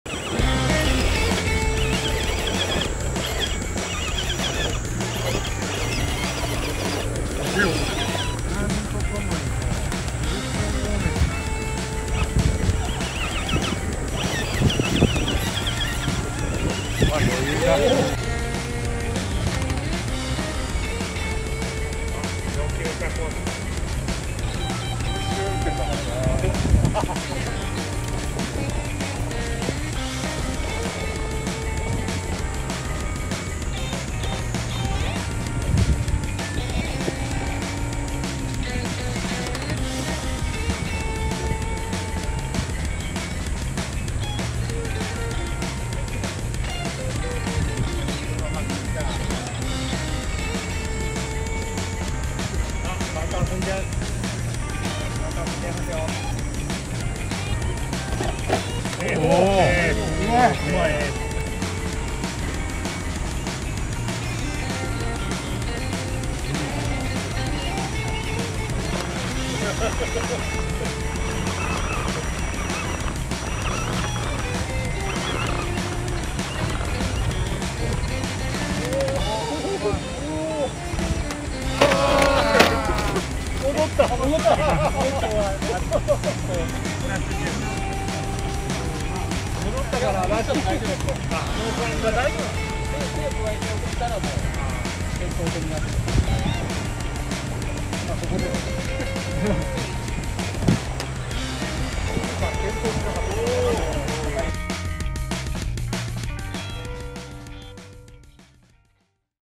TOKYO RC COMPETITION - AXIAL SCX10 ii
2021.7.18
東京の御岳渓谷でのユルいコンペの抜き出し動画。
本編では時間の都合で大幅カットしていますが、コチラはほぼノーカットです。